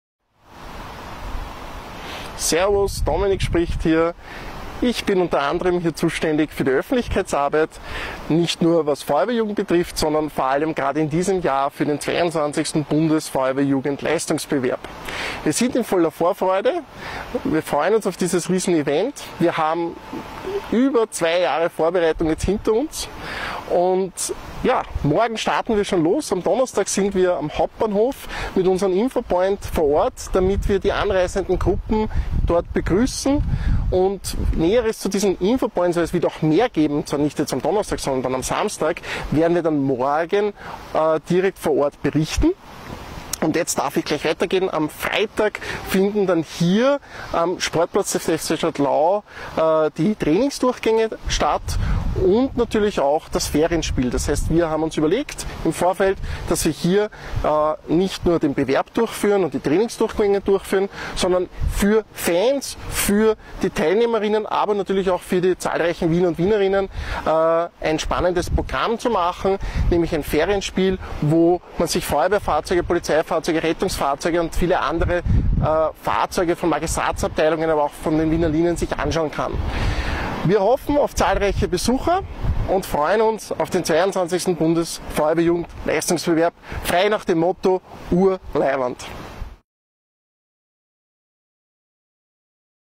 BFJLB 2018 - Was passiert in den nächsten Tagen am 22. BFJLB?
Der Leiter unserer Öffentlichkeitsarbeit, Dominik Zeidler erklärt euch was in den nächsten Tagen alles geplant ist! Morgen kommen die ersten Bewerbsgruppen bereits nach Wien! Ur leiwand!